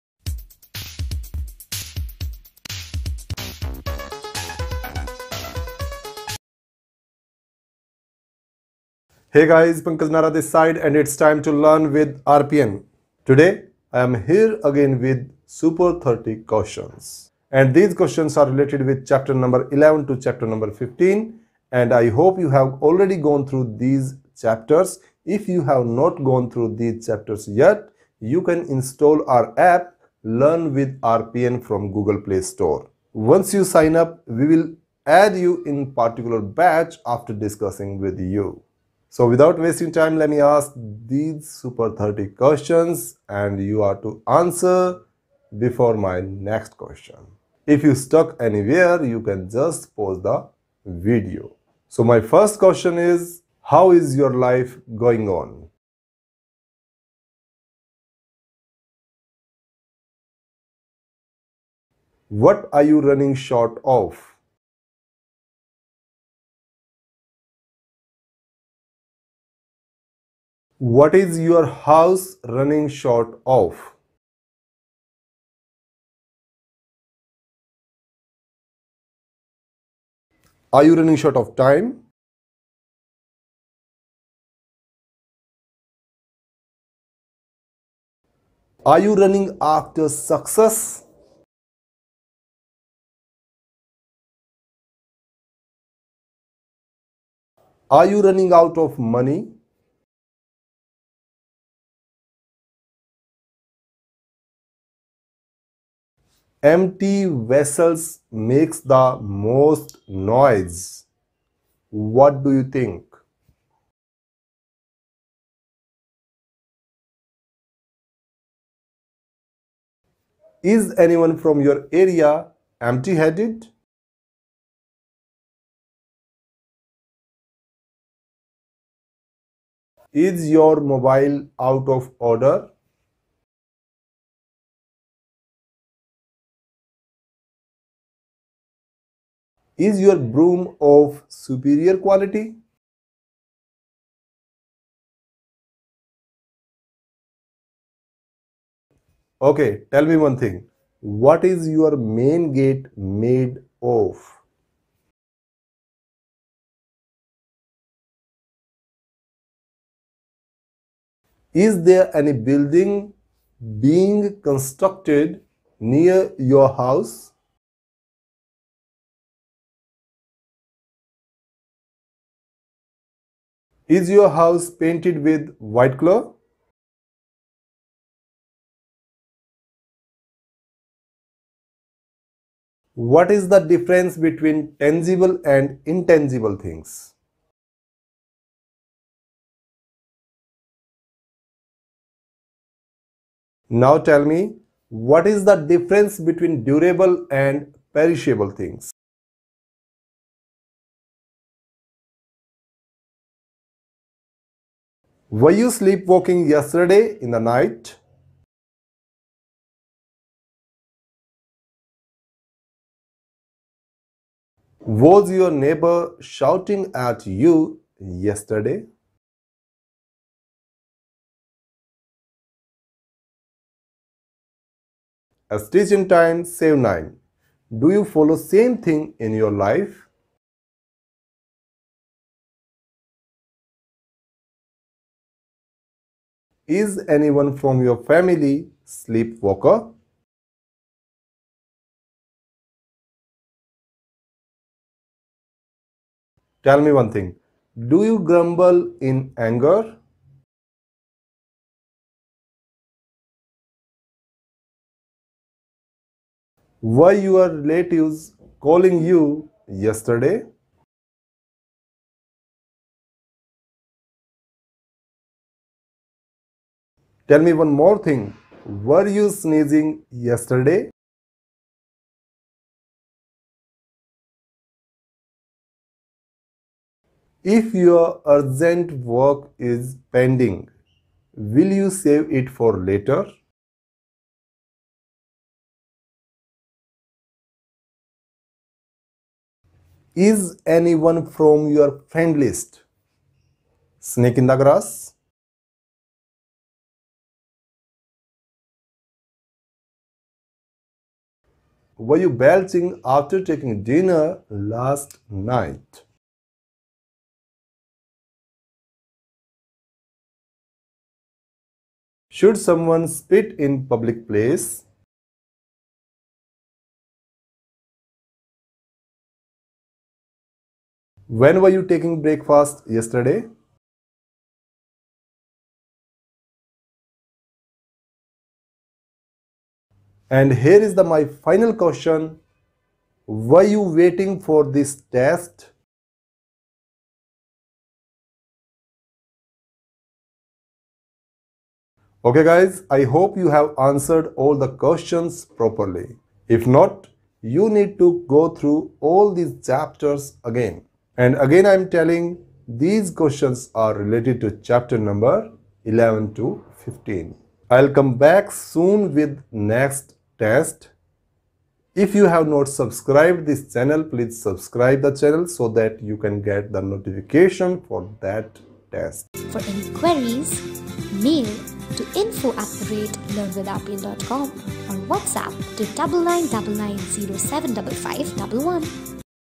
English Spoken Course | Test 4 - super30 Questions by Pankaj Nara
NOTE: Please answer with complete sentence. Answer in “YES” and “NO” will not be accepted.
1. How is your life going on?
2. What are you running short of?
3. What is your house running short of?
4. Are you running short of time?
5. Are you running after success?
6. Are you running out of money?
7. “Empty vessels makes the most noise” What do you think?
8. Is anyone from your area empty headed?
9. Is your mobile out of order?
10. Is your broom of superior quality?
11. What is your “main gate” made of?
12. Is there any building, being constructed near your house?
13. How many rooms are there in your house or flat?
14. Is there any LED in your house?
15. Is your house painted with while color?
16. What is the difference between tangible and intangible things?
17. What is the difference between durable and perishable things?
18. Were you sleepwalking yesterday in the night?
19. Was your neighbour shouting at you yesterday?
20. “A stich in time save nine”. Do you follow same thing in your life?
21. Is anyone from your family sleepwalker?
22. Do you grumble in anger?
23. Were your relatives calling you yesterday?
24. Were you sneezing yesterday?
25. If your urgent work is pending, will you save it for later?
26. Is anyone from your friend list, snake in the grass?
27. Were you belching after taking dinner last night?
28. Should someone spit in public place?
29. When were you taking breakfast yesterday?
30. Were you waiting for this test?

Note: If you have passed this test, please wait for next test else watch all the videos of playlist related with English Speaking course in our channel.